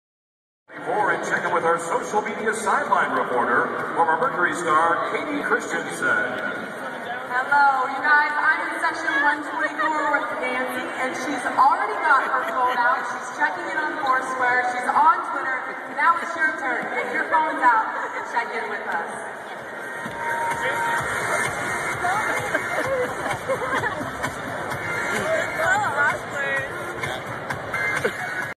Twitter twit!
Nancy, working it at the Suns vs Warriors game! (1/3/2012)